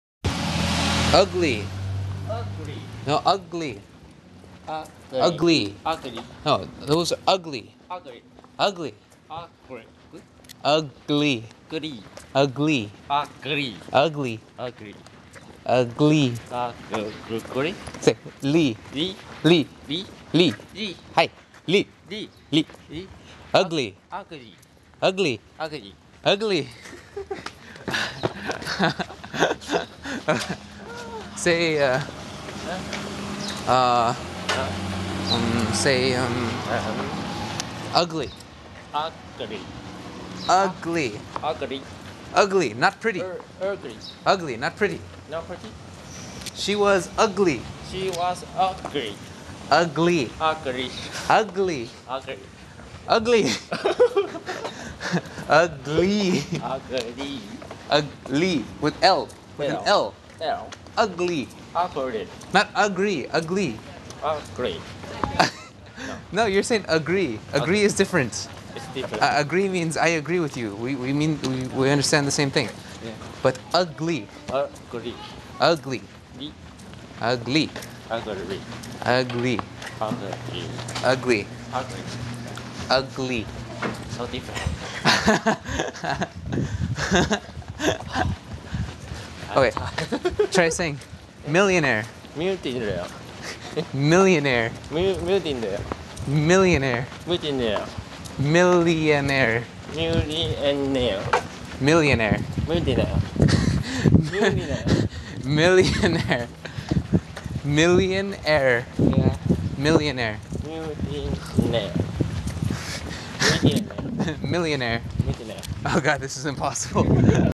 teaching the concept of ugly...
kho has some problems pronouncing his "L"s lol so i was trying to tech him... it didnt work too well hahahahaha